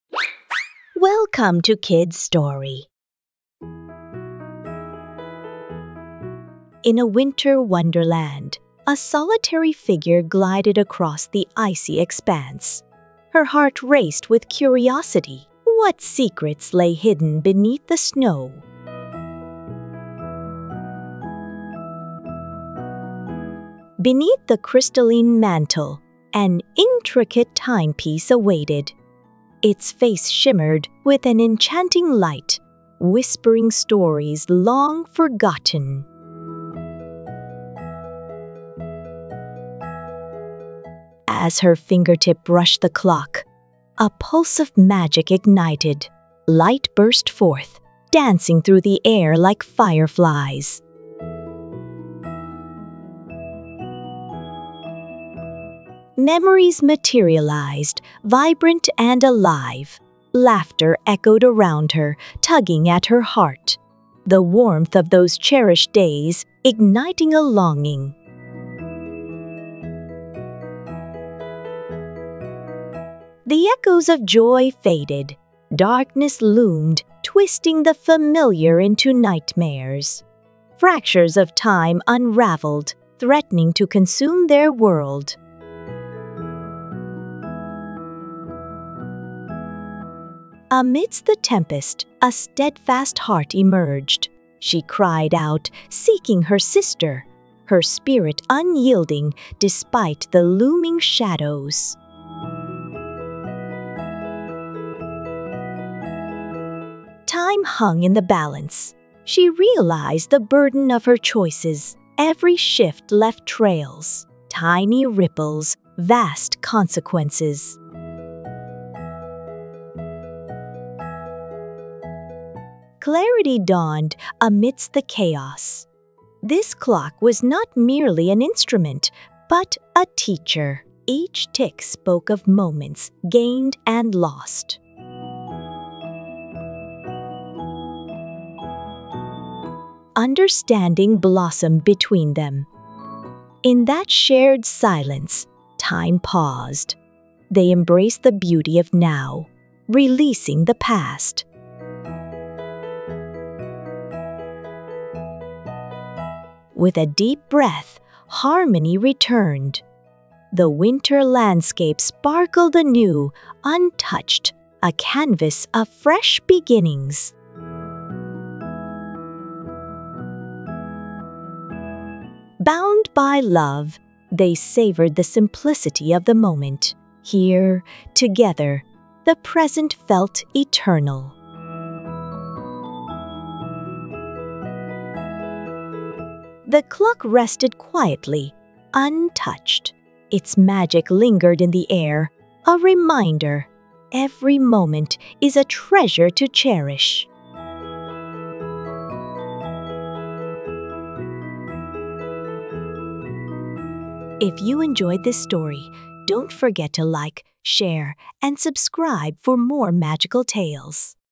Elsa's Time-Traveling Adventure: A Tale of Time and Friendship | Fairy Tales | Bedtime Stories
Join Princess Elsa on a magical adventure as she discovers an ancient clock with the power to manipulate time. When a mysterious force threatens to disrupt the flow of time, she must use her courage and wisdom to restore balance and protect the future. This heartwarming tale is filled with wonder, friendship, and the importance of cherishing every moment.